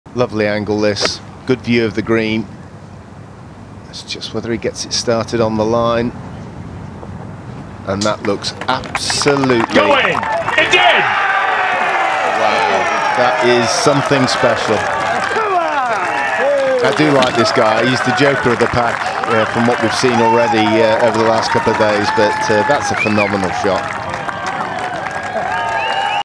INCREDIBLE EAGLE at The Walker Cup 🦅
John Gough holes out for eagle at the 6th hole during The Walker Cup in St Andrews

The R&A | The Open | The AIG Women's Open